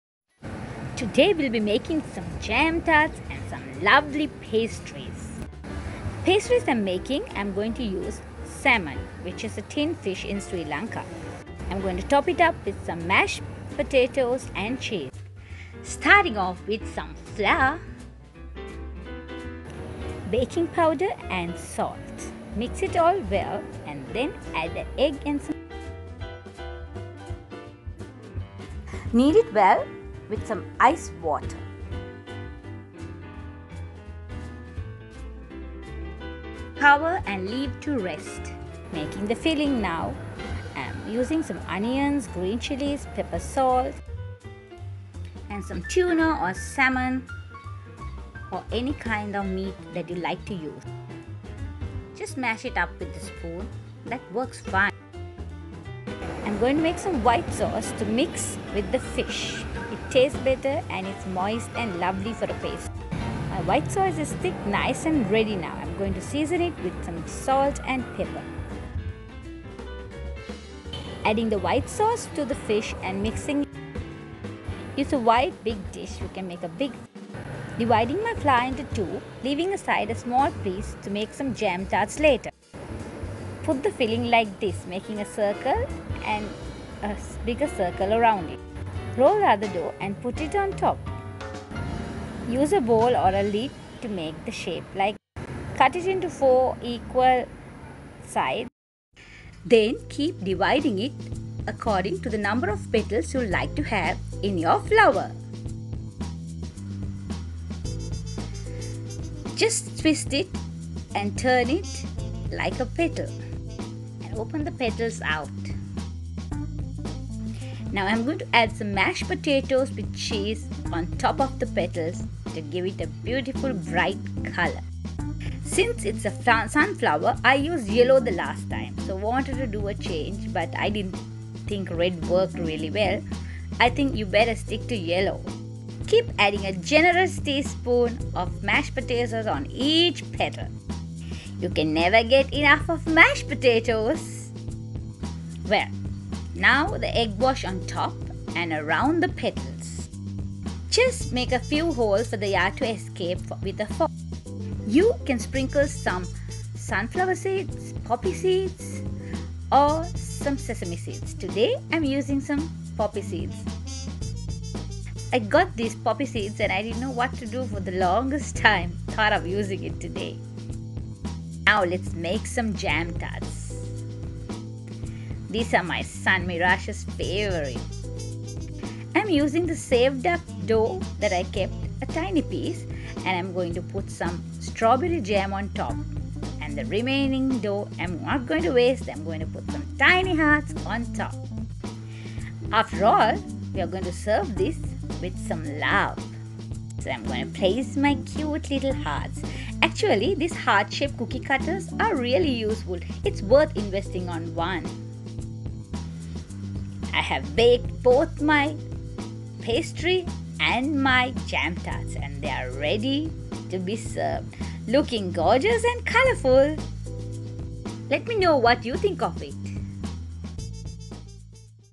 Fish and Potato Flower Pastry / Jam Tarts
Its a shortcrust pastry made in a shape of a flower, I added some mashed potatoes with Cheese to make it Yum and added some colour, last time i used Yellow because it looks like a sunflower, it looked pretty.  Today i just tried red but i found it way too colourful:) you may want to try it with No colour or some egg yellow colouring.
i used tin fish (salmon) today , you may use any filing you like the attraction is the shape. the white sauce gives it moisture and a rich taste, i like the way white sauce enhances the Shortcrust pastry. with the remaining pastry i made a few of my sons favorite Jam tarts. they were awesome. enjoy my flower-power:)  and Jam tarts with Love.